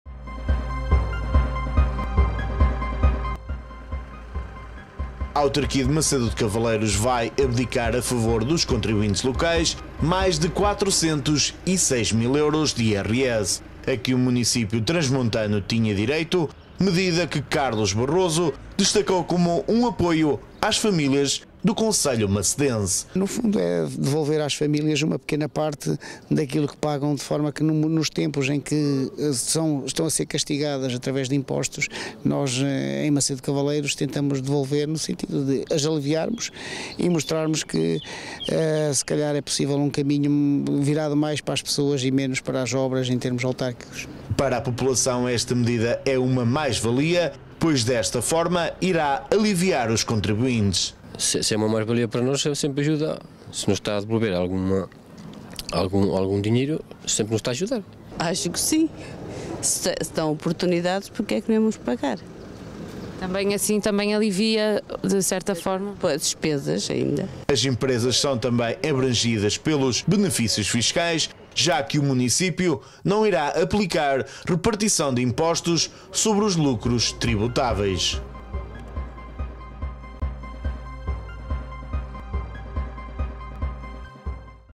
AUTARQUIA DE MACEDO DE CAVALEIROS VAI ABDICAR DO IRS DOS CONTRIBUINTES LOCAIS
Macedo de Cavaleiros abdica de 406 mil euros de IRS para os contribuintes esta medida é realizada à 3 anos pela autarquia macedense.